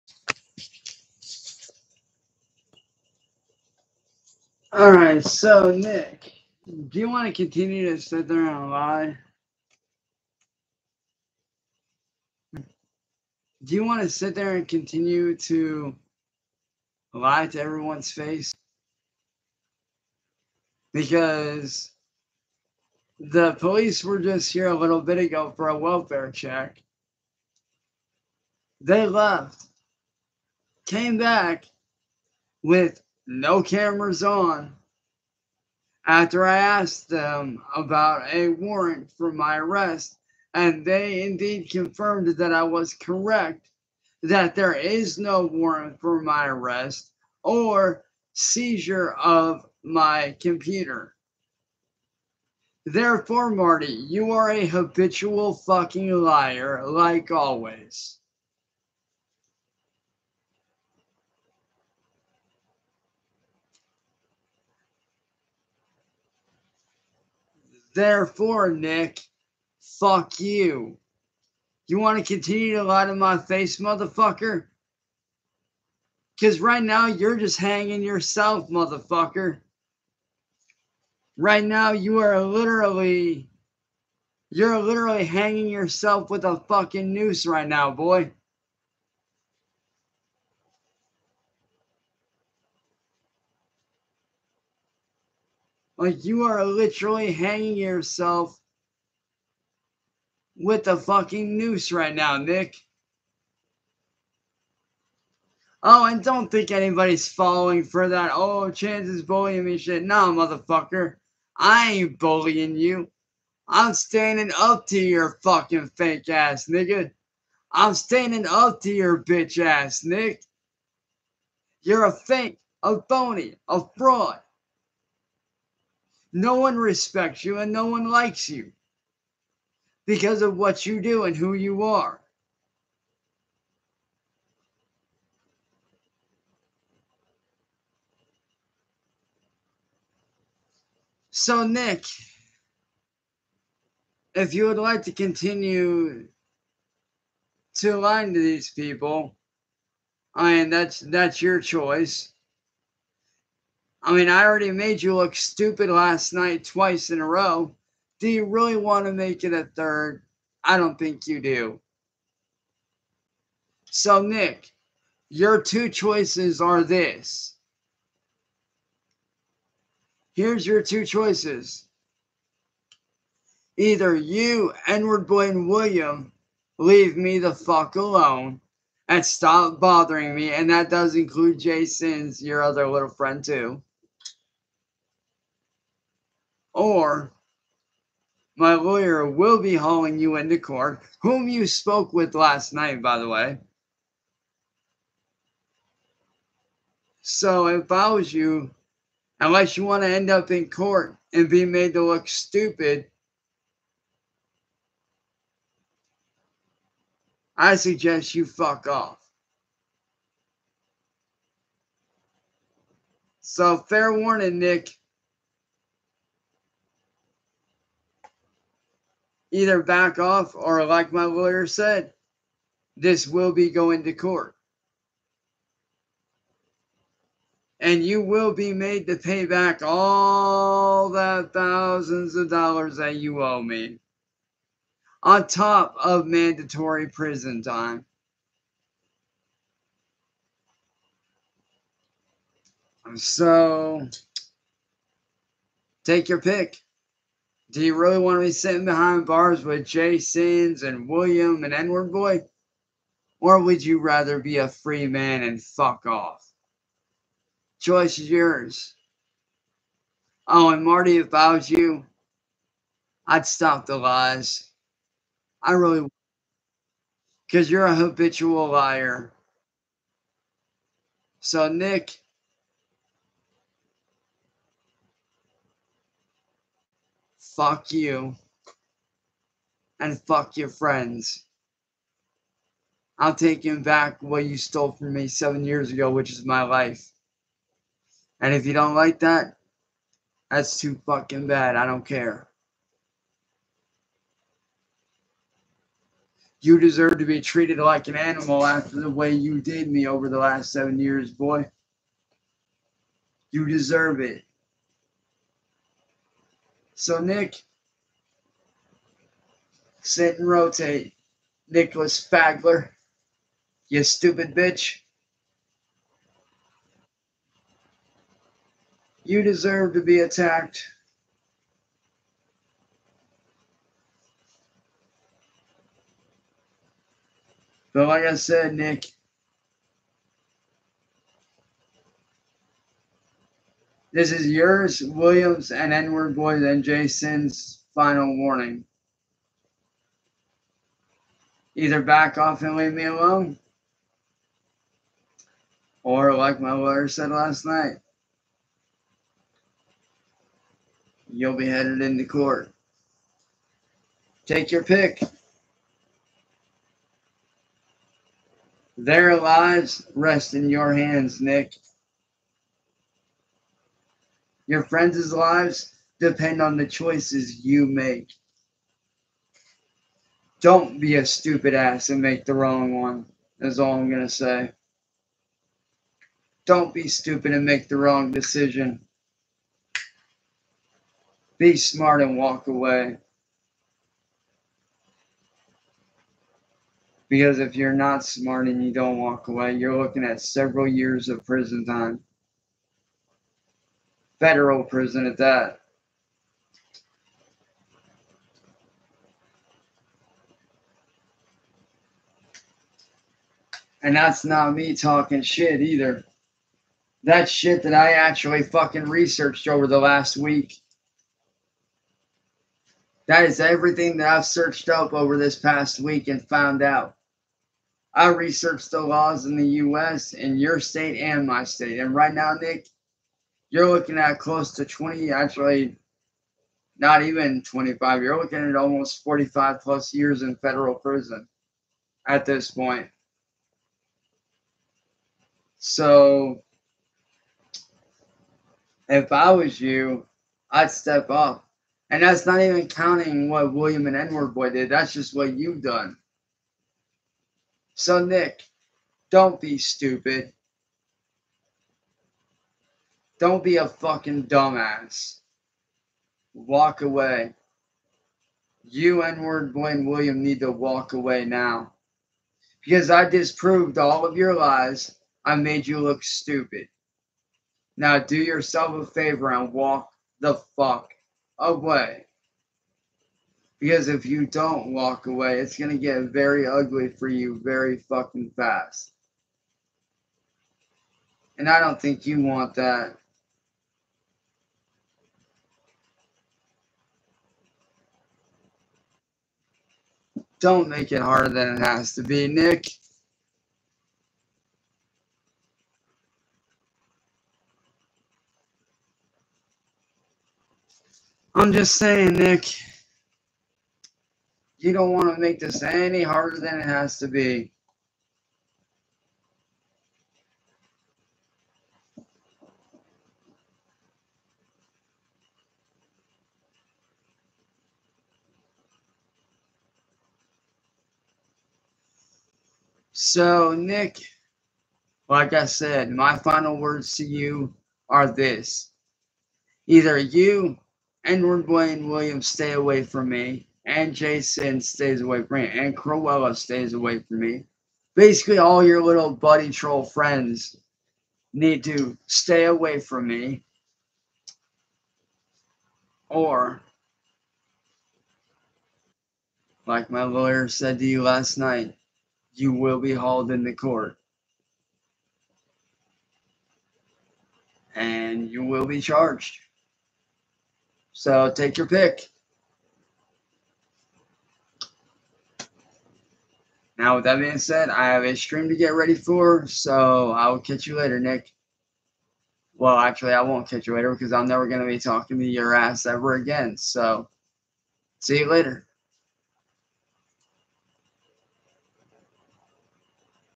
Cyraxx Live on YT. "THERE IS NO WARRENT !!!!!!!!!!!".6/6/2024. Lies and excuses from the bully.
Streamed 7 hours ago.